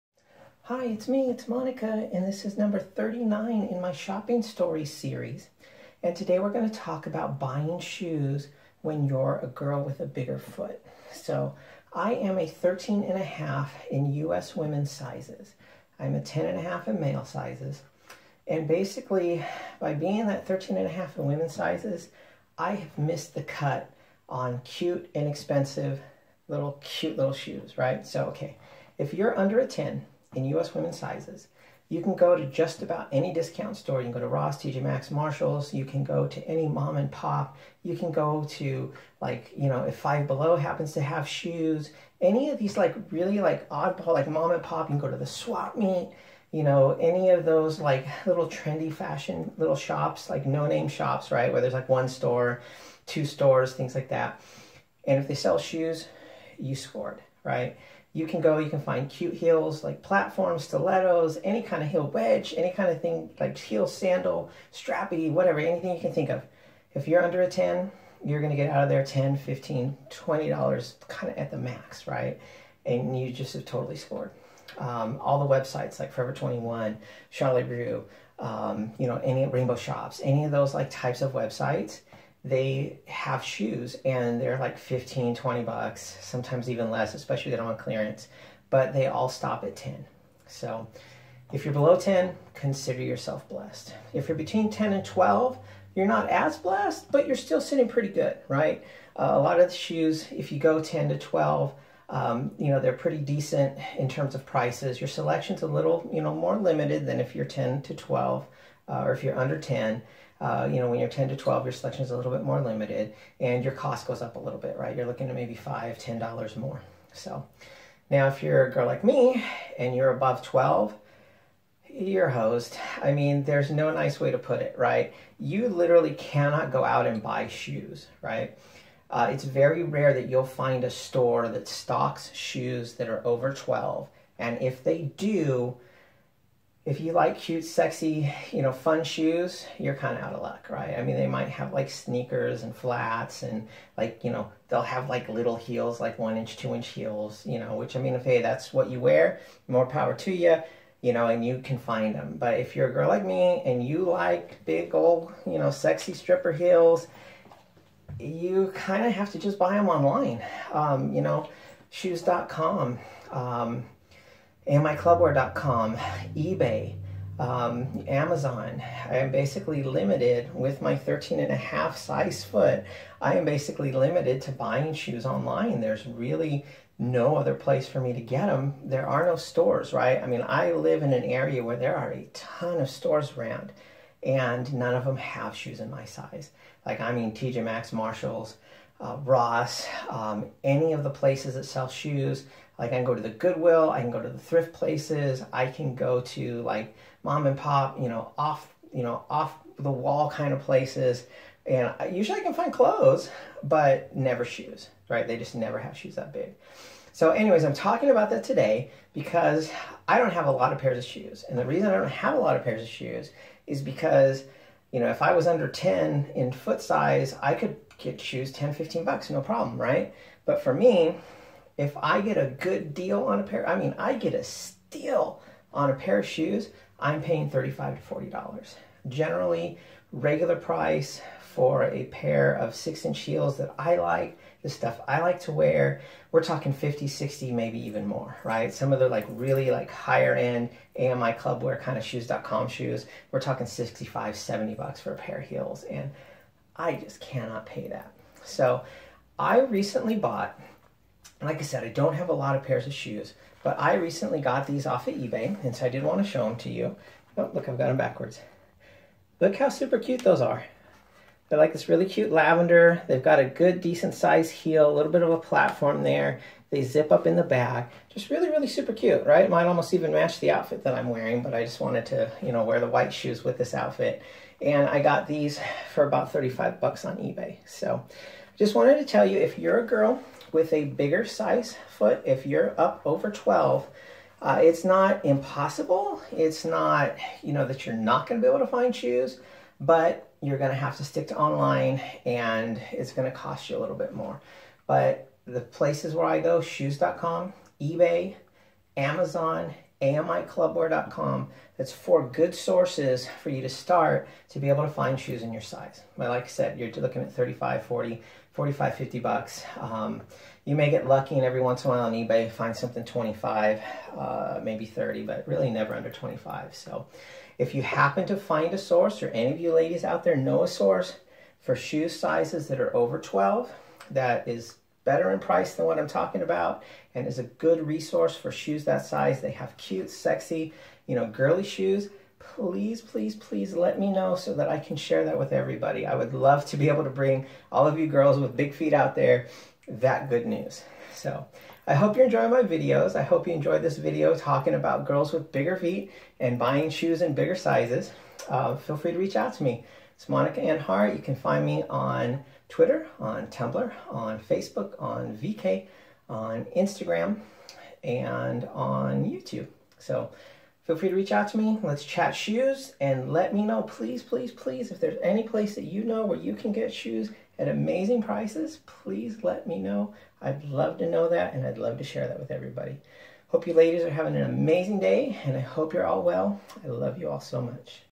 Shopping Stories #39 - Buying Shoes When You Have Bigger Feet
Talking all about the difficulty we girls with bigger feet can have in finding cute, sexy shoes!...